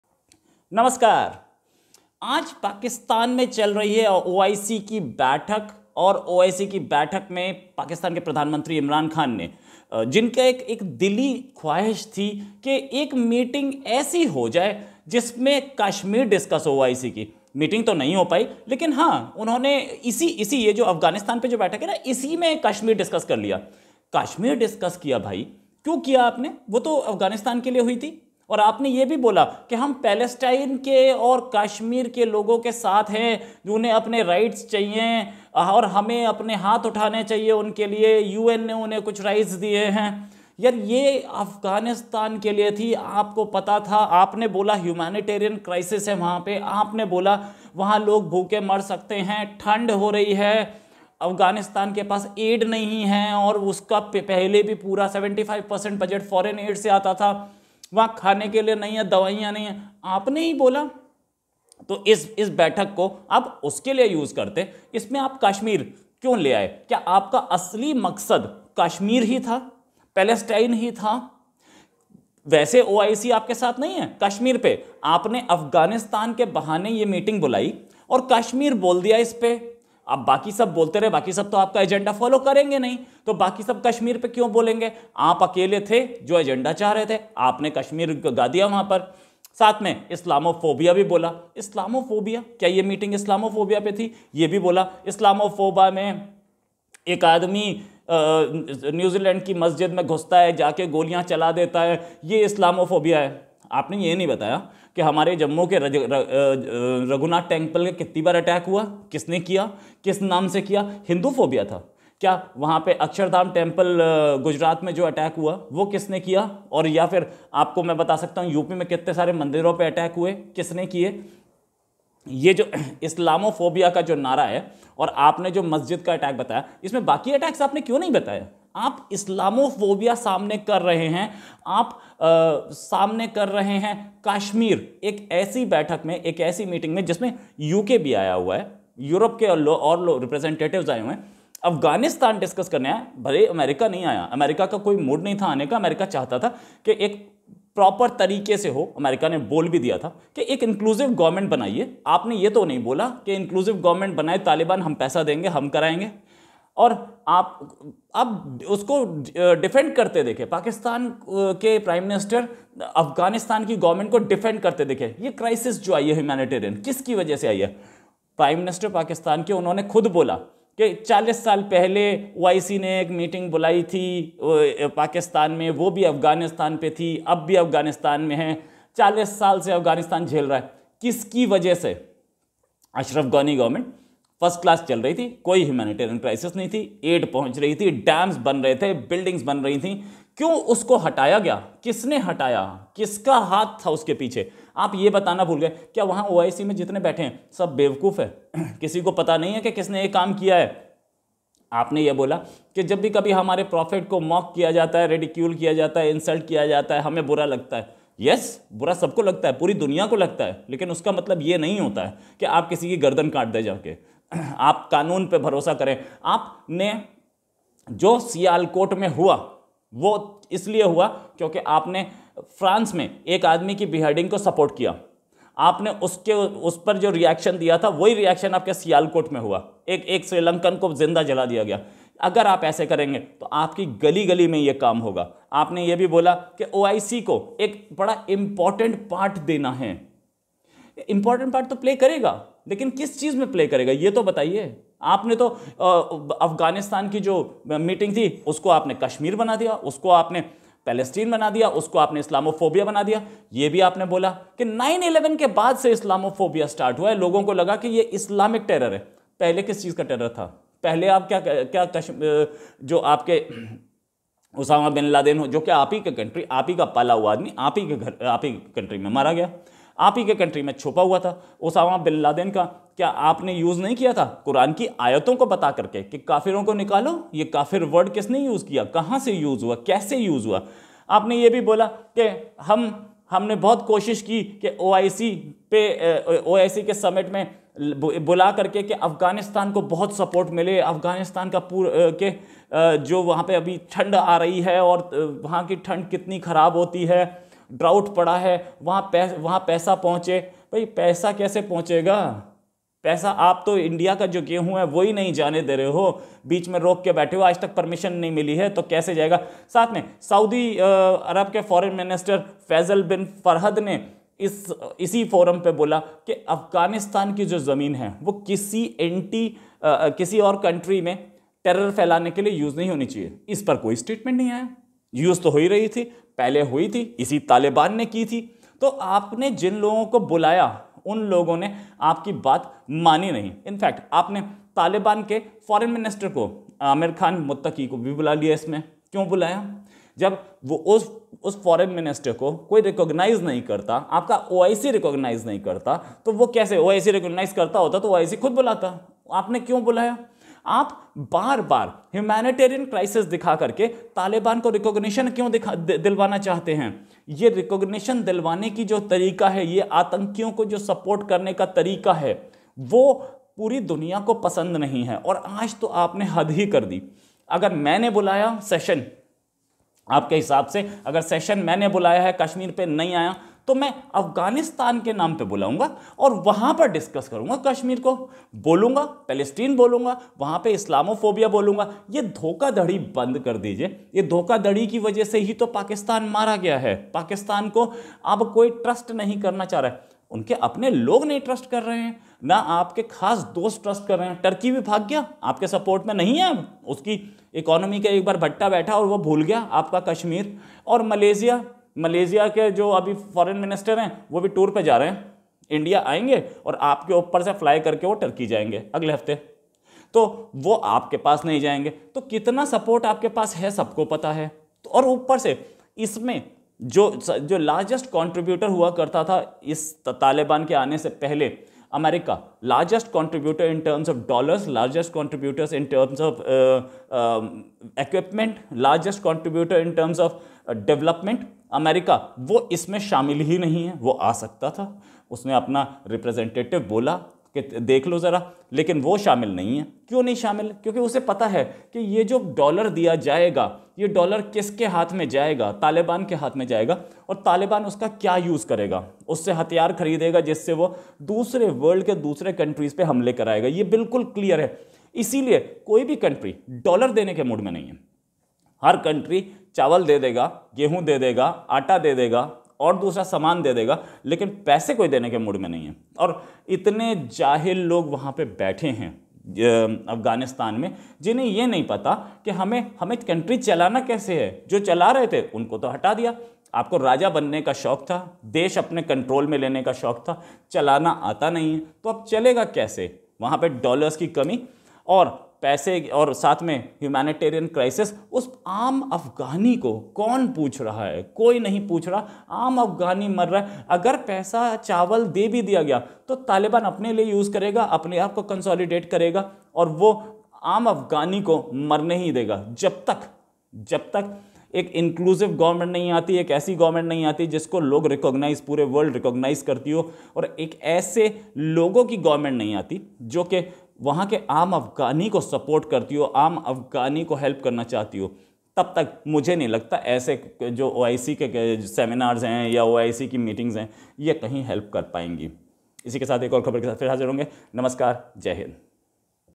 Imran misuses OIC forum on Afghanistan. निकला इमरान के दिल का दर्द Pak Media on India Latest
Imran misuses OIC forum on Afghanistan. निकला इमरान के दिल का दर्द

Rohit Sharma